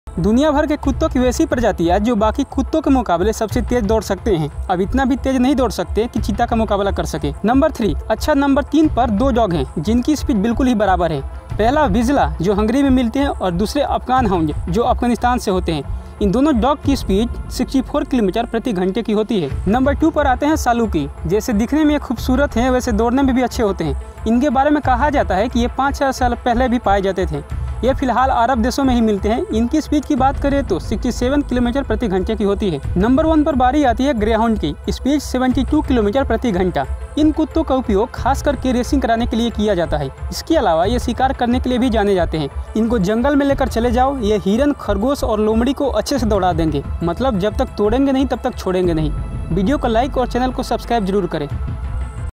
दुनिया के 3 सबसे तेज दौडनेवाले कुत्ते । Top 3 Fastest dog breeds in the world
world most dangerous animals
pet lovers status
dog market in india
saluki dogs speeds
greyhound racing